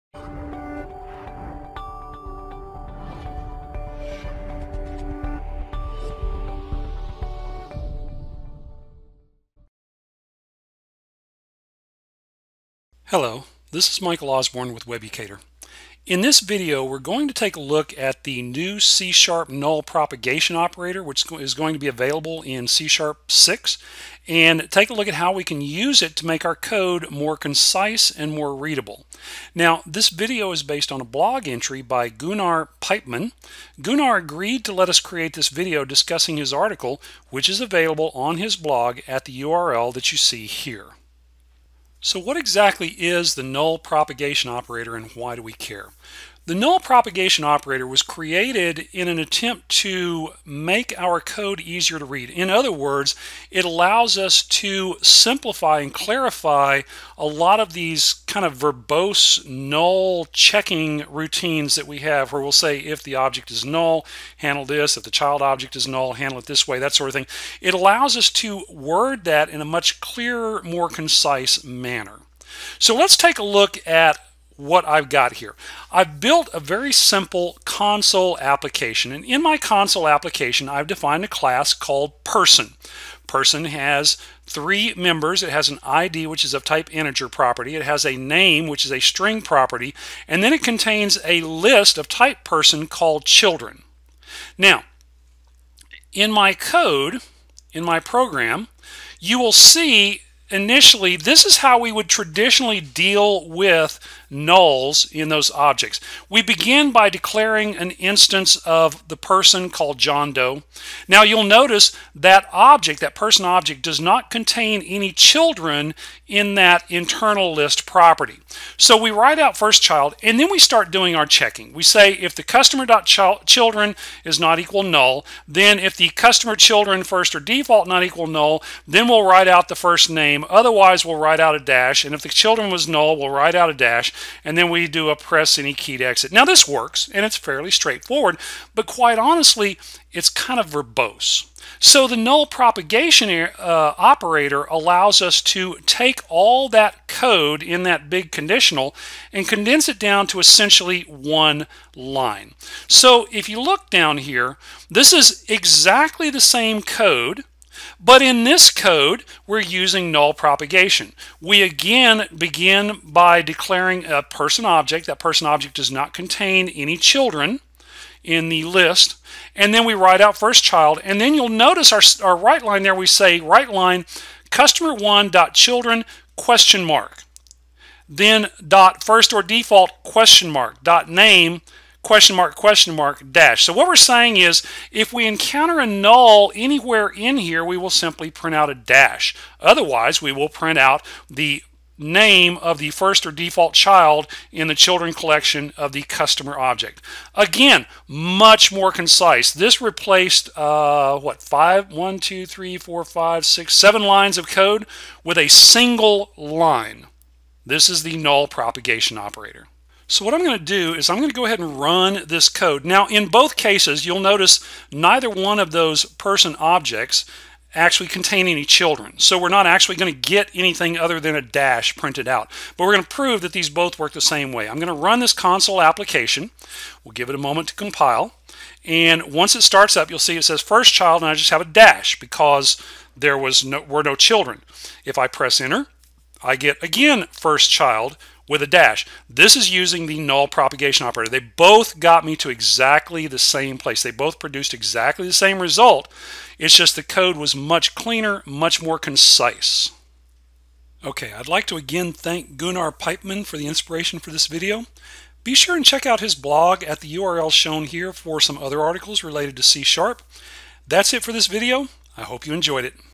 C# Null Propagating Operator
In this video, we present the C# null propagation operator and show how to use it to make code more concise and readable.  The video was inspired by a blog post by Gunnar Peipman